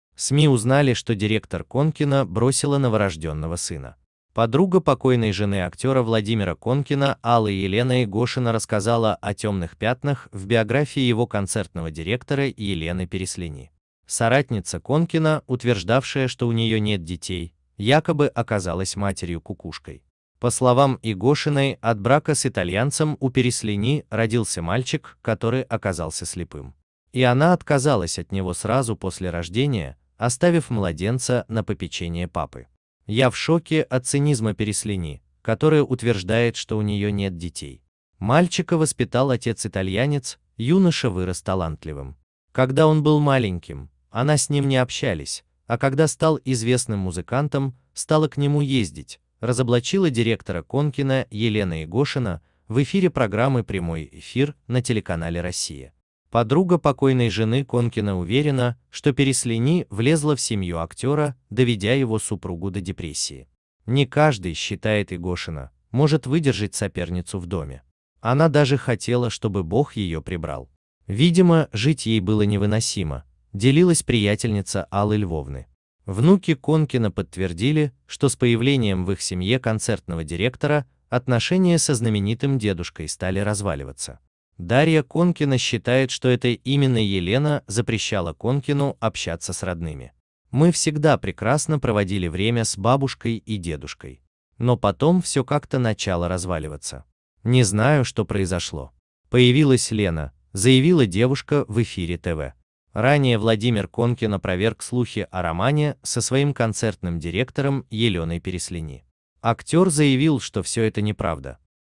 Друзья Конкиных раскрыли правду о директоре актера!
новости шоубизнеса
владимир конкин
софия конкина
бывший муж конкиной

Свежие новости из мира шоубизнеса произошли сегодня !Подруга покойной жены актёра Владимира Конкина Аллы Елена Игошина рассказала о тёмных пятнах в биографии его концертного директора Елены Переслени. Соратница Конкина, утверждавшая, что у неё нет детей, якобы оказалась матерью-кукушкой. дочь конкина
Так что же произошло и что случилось у Конкиных? По словам Игошиной, от брака с итальянцем у Переслени родился мальчик, который оказался слепым. И она отказалась от него сразу после рождения, оставив младенца на попечение папы.
Тайна конкиных раскрыта! Я в шоке от цинизма Переслени, которая утверждает, что у неё нет детей. Мальчика воспитал отец-итальянец, юноша вырос талантливым. Когда он был маленьким, она с ним не общалась, а когда стал известным музыкантом, стала к нему ездить, — разоблачила директора Конкина Елена Игошина в эфире программы «Прямой эфир» на телеканале «Россия». Новости 20 октября. Новости сегодня
свежие новости из мира шоубизнеса сегодня

Подруга покойной жены Конкина уверена, что Переслени «влезла в семью» актёра, доведя его супругу до депрессии. Не каждый, считает Игошина, может выдержать соперницу в доме.
Она даже хотела, чтобы Бог её прибрал. Видимо, жить ей было невыносимо, — делилась приятельница Аллы Львовны.